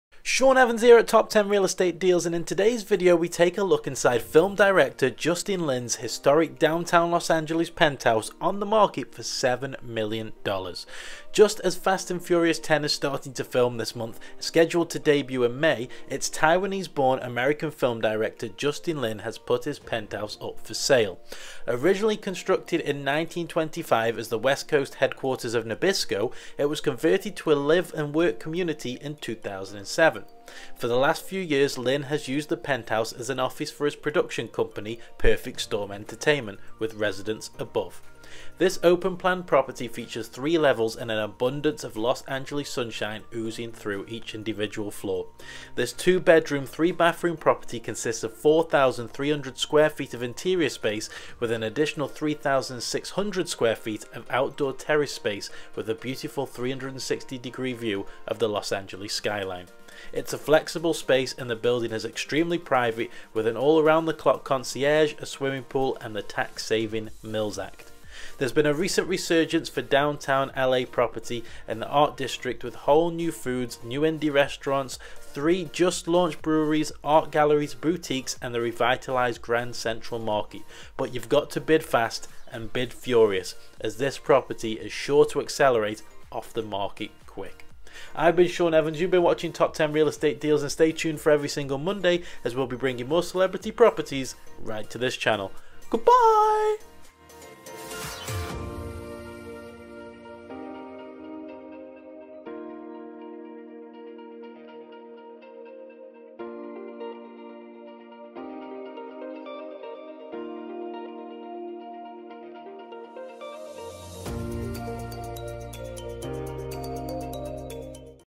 Inside Justin Lin's $7 Million Downtown LA Penthouse
Just as Fast & Furious 10 is starting to film this month, scheduled to debut in May, its Taiwanese-born American film director, Justin Lin, has put his historic Downtown Los Angeles penthouse on the market, located in the thriving LA Arts District.

Justin Lin thumbnail photo credit - s_bukley / shutterstock.com
Property Photos credit - NeueFocus